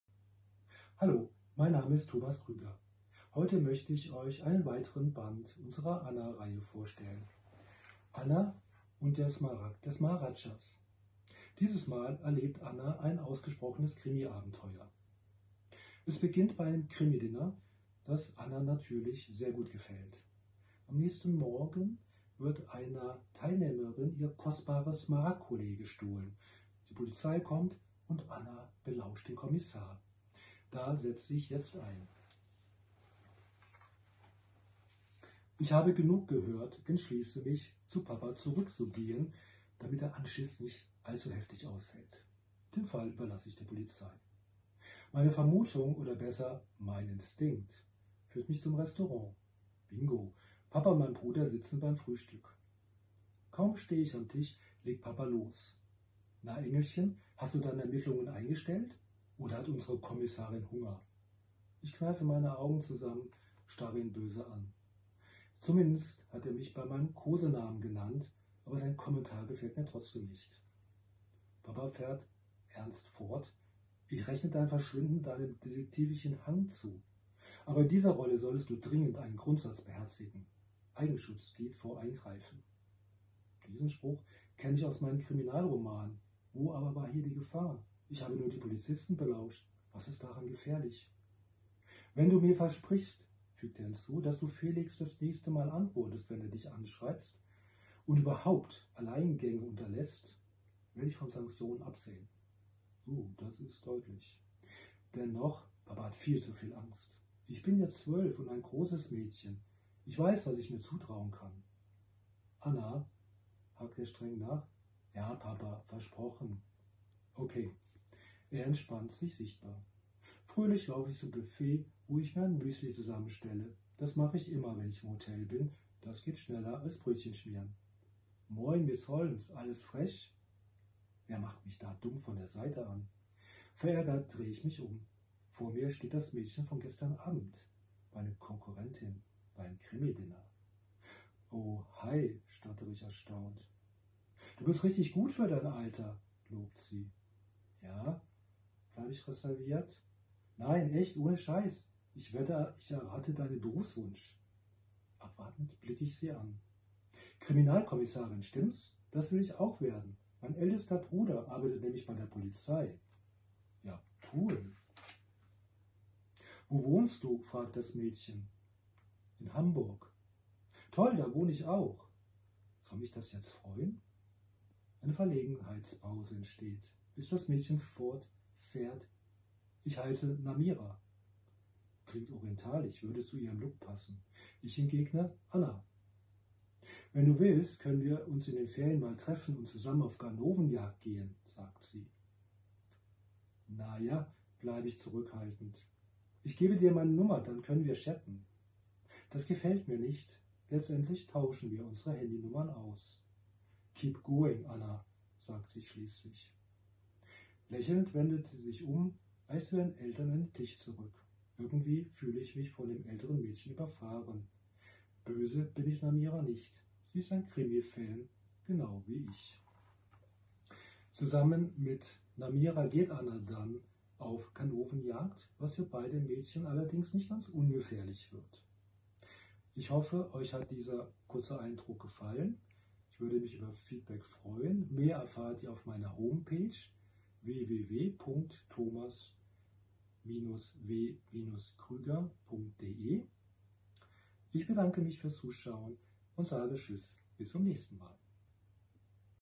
Bright (K)night [Thomas W. Krüger Buchempfehlung] | Collectables | Part 5 ANNA-Series
Anna und der Saphir von Burg Drachenfels
Ein Wochenendausflug führt Familie Stührmann mit ihren drei Kindern auf Burg Drachenfels bei Bonn. Am Morgen im Hotel geschieht das Unfassbare: Anna, die jüngste Tochter, ist spurlos verschwunden. Selbst die Polizei steht vor einem Rätsel. Wo ist Anna? In ihrer Welt verschwimmen Realität und Traum. Sie findet sich im Jahr 1269 wieder, mitten auf die Baustelle des Kölner Doms. Dort erlebt Anna ein fantastisches Abenteuer in einer Zeit, die ihr völlig fremd ist und voller Gefahren steckt.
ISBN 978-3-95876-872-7

Anna und der Rubin der Finsternis
Während der Sommerferien reist Familie Stührmann mit ihren drei Kindern nach Transsilvanien. Bereits am ersten Tag geraten sie in Not, als ihr Wagen wegen eines Unwetters von der Straße abkommt. Sie finden Schutz in einer alten Burg, deren Bewohner sich mehr als sonderbar verhalten. Zu allem Überfluss verliebt sich Annas Bruder in die verführerische Tochter der Burgbesitzer. Kann Anna die unheimlichen Ereignisse aufklären und das Geheimnis der Burg und seiner Bewohner enträtseln?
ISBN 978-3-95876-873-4

Anna und der Smaragd des Maharadschas
Zu ihrem 12. Geburtstag erhält Anna ein besonderes Geschenk: Ihr Vater lädt sie zusammen mit ihrem Bruder auf einen Gutshof zu einem Krimidinner-Wochenende ein. Dort erwartet die Geschwister eine Theatergruppe, die sie mit einer spannenden Darbietung unterhält. Als einem der Gäste ein wertvolles Smaragdcollier gestohlen wird, fühlt Anna sich voll in ihrem Element als Detektivin. Sie beginnt mit eigenen Nachforschungen, doch gerät dabei alsbald in große Gefahr.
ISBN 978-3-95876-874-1

Anna und der Diamant des Winterlandes
Die Weihnachtsferien verbringt Familie Stührmann mit ihren drei Kindern im Allgäu. Bei einem Museumsbesuch findet Anna einen Diamanten, den sie für eine wertlose Nachbildung hält. Schon bald ereignen sich seltsame Dinge in ihrer Umgebung: Tiere können mit ihr sprechen, Menschen verwandeln sich in Märchengestalten. Ein Besuch auf Schloss Neuschwanstein stellt schließlich die Welt auf den Kopf: Der Winter ist verschwunden. Kann Anna die Natur retten?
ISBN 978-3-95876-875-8

Thomas W. Krüger wurde 1966 in Koblenz geboren und lebt mittlerweile seit über 20 Jahren im Westerwald bei Montabaur. Nach dem Gymnasium absolvierte er eine Fachausbildung zum Informatiker IHK und arbeitet seitdem als EDVSpezialist im Rhein-Main-Gebiet. Seine Leidenschaft zum Schreiben begann am Ende der Schulzeit mit ersten Krimi-Kurzgeschichten für den familiären Kreis. Neben einer fesselnden Romanhandlung ist es ebenso ein Anliegen des Autors, unsere Historie einmal unter einem anderen Aspekt einem breiten Leserkreis näher zu bringen. Ohne die üblichen Zwänge der Schulwissenschaft werden Fakten und Thesen wertfrei gegenüber gestellt und vermitteln so ein interdisziplinäres und umfassendes Bild der Ereignisse.

Ihr wollt mehr über Thomas wissen?
Sprecht ihn einfach direkt an!

WE BRIGHTON® YOUR DAY mit Werken von Thomas W. Krüger